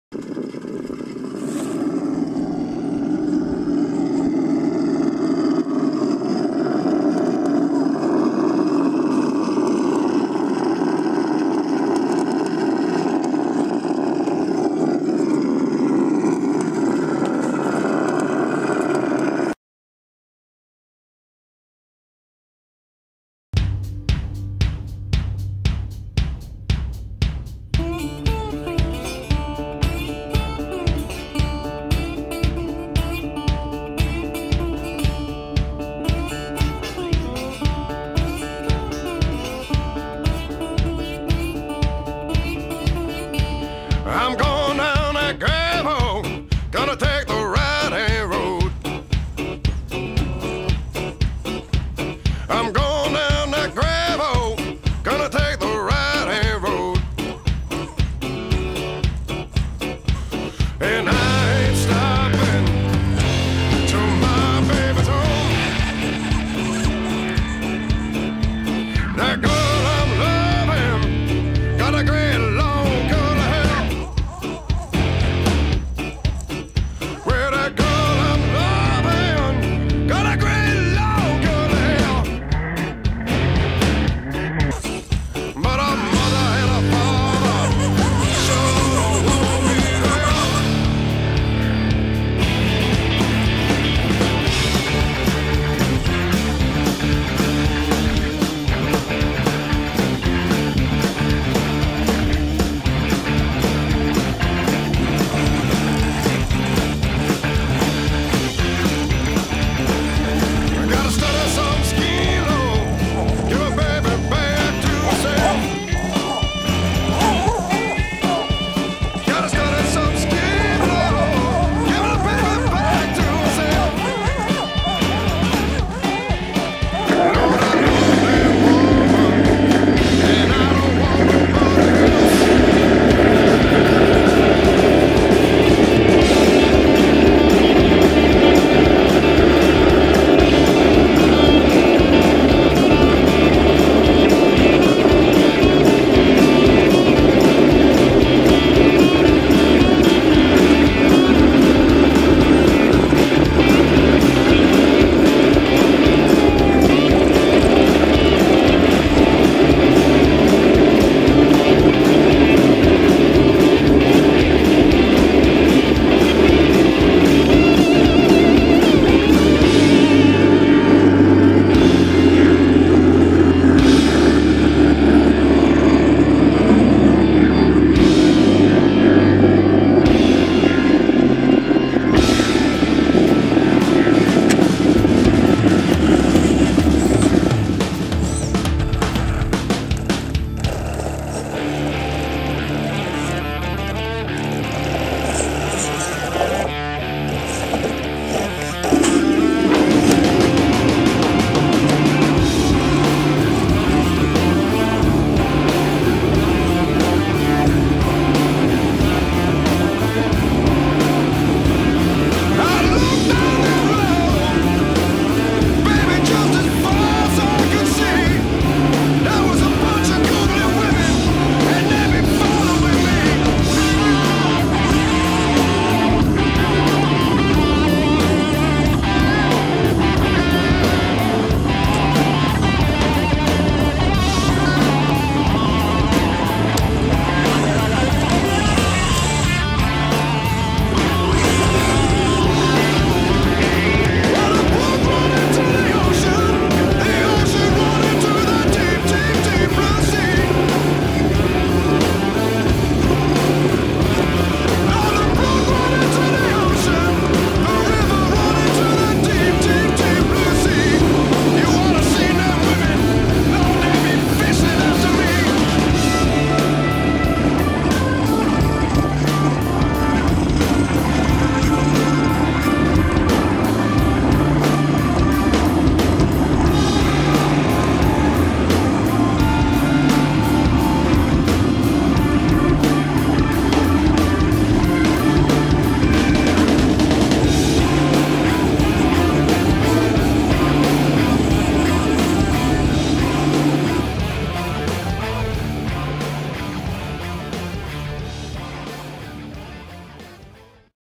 Axial SCX10 Scale trail run at Beacon Heights PT2
2 Axial SCX10s doing a scale run up Beacon Heights trail.